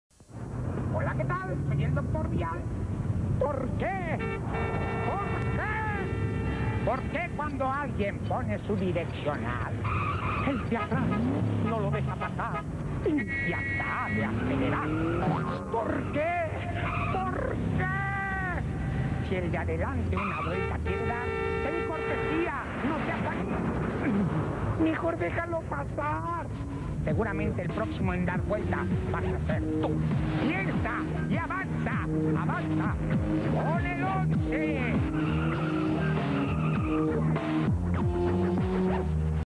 Capsula Once Tv 2002-2003 2
Capsula de Once Tv sobre la educación al manejar en via publica